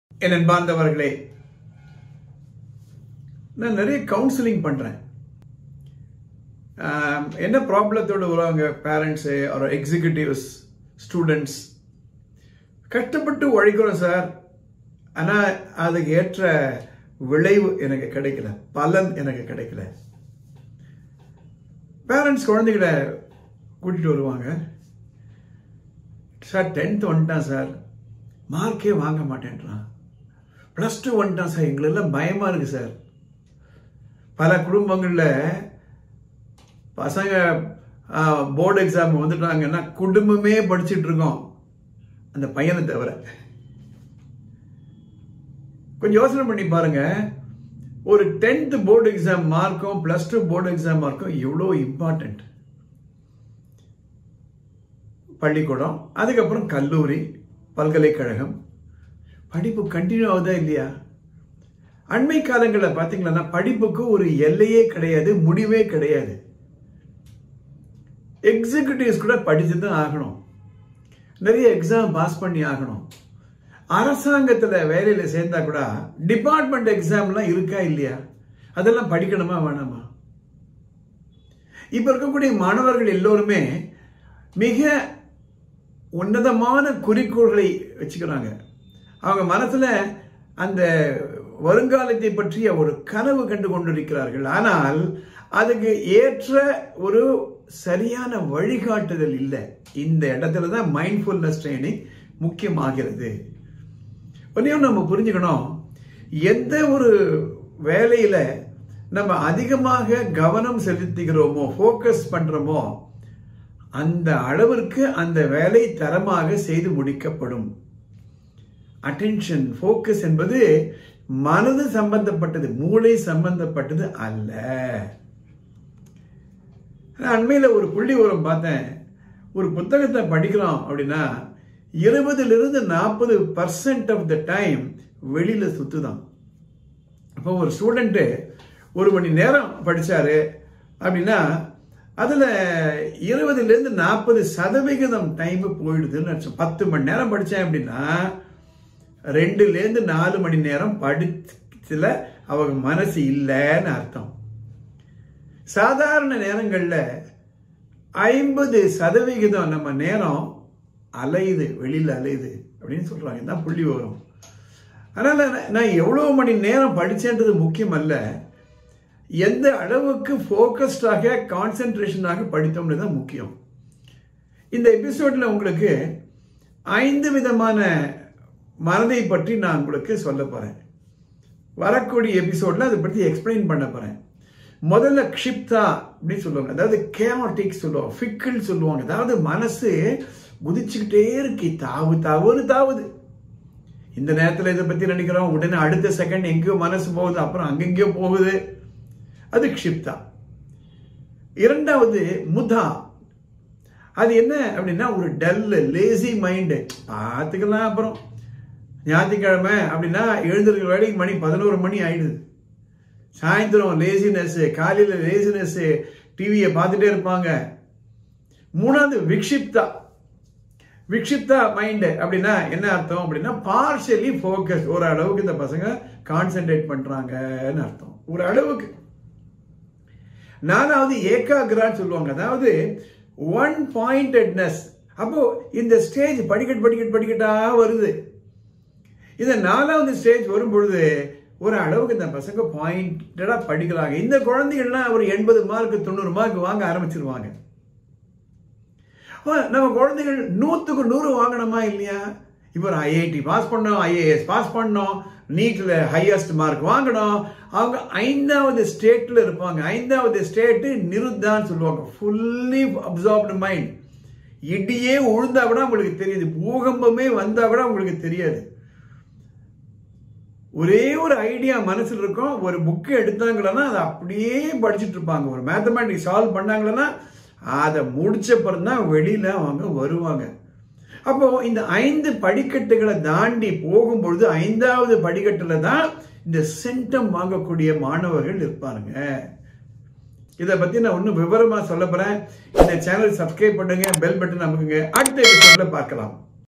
Mindfulness Trg in Tamil Part 4 : Five types of mind!
In a normal day, our mind wanders about 50% of the time; when we read a book, our mind wanders about 20 to 40 % of the time. We should arrest our wandering mind.